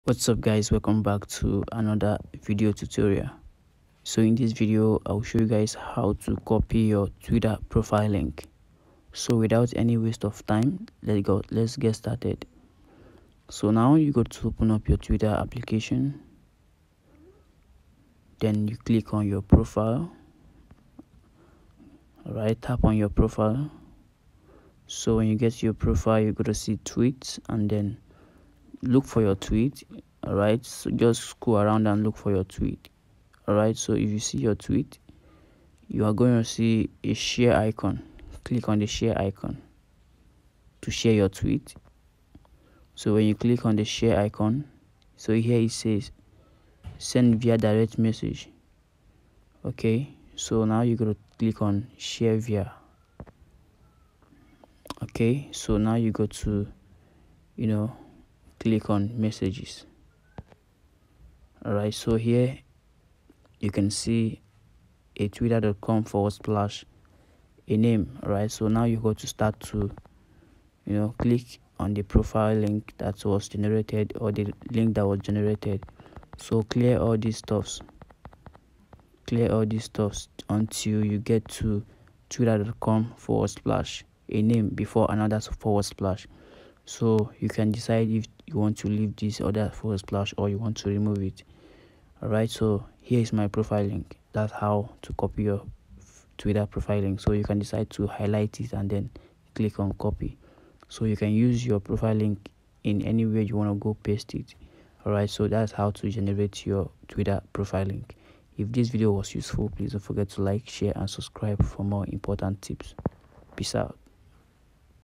How To Copy Twitter Profile Link
In this video I will show you how to copy your Twitter profile link or how you can get it in other way. It's really easy and it will take you less than a minute to do so!